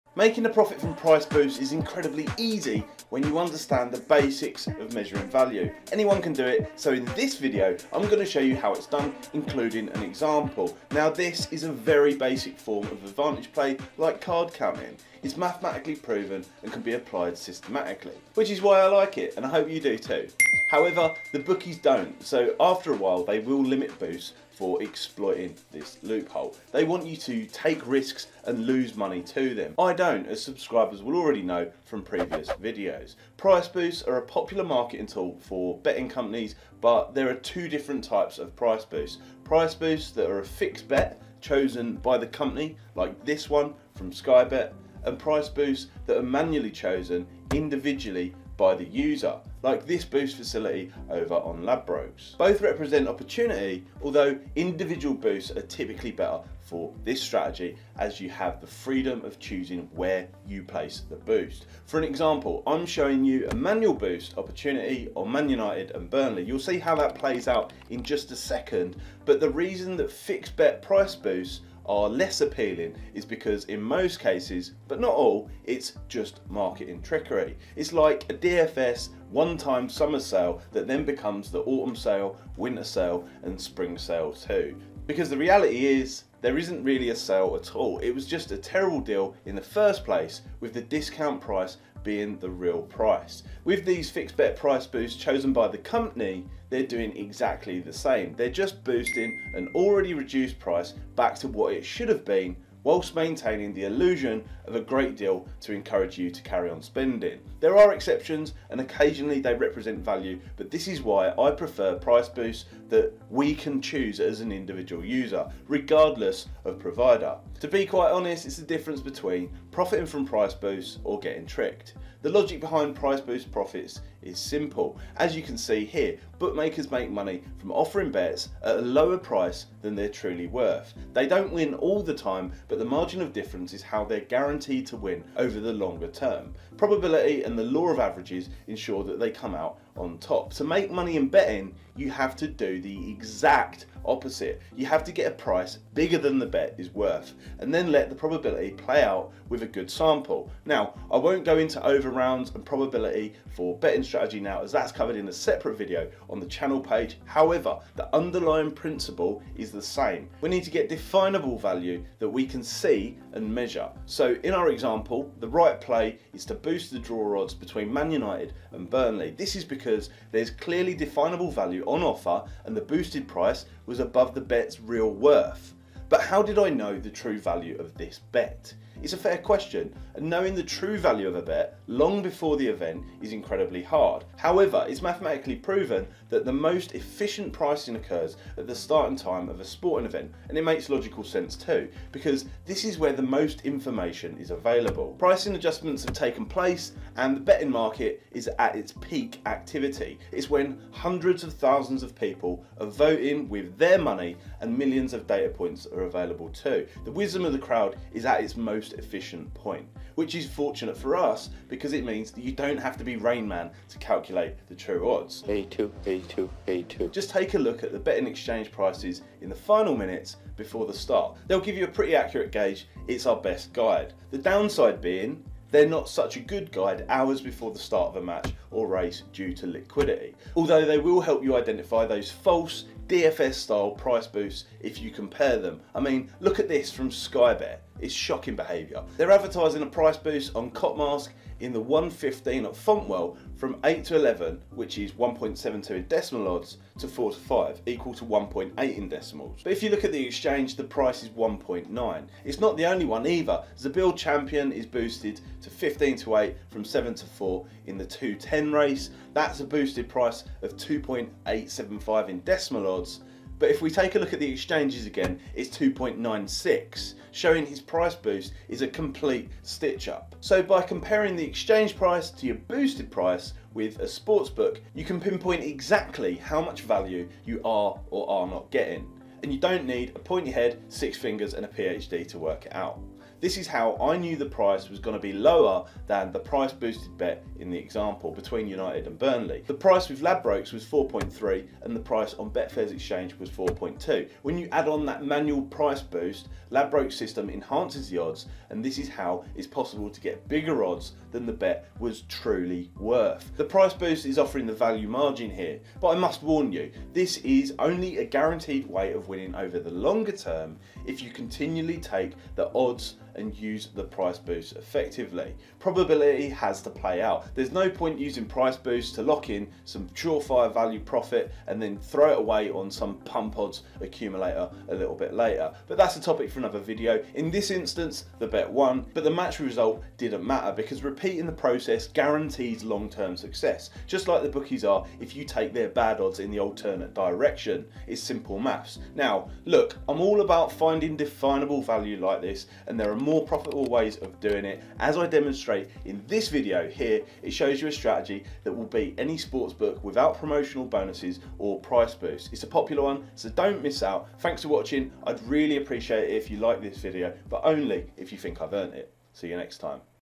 PRICE BOOSTS: Unleash Football Betting Profit With This Trick
Watch this price boost betting strategy that works on football and other sports with well-known price boost providers like Skybet. By using the price boost facility it was possible to lock in a guaranteed profit on the Man Utd v Burnley match this week. Take a look as I explain in simple terms...

By carefully selecting the right situations, as explained, it's possible to stack the odds in your favour. Manchester United v Burnley last night was one of those situations. Football is a great place to apply this betting strategy as boost providers offer competitive odds most of the time. Keep an eye on sky sports football over the next couple of weeks as this will be happening a lot.

License ID: 4z0P6QpV95M

This video shows a price boost betting strategy that works on Football.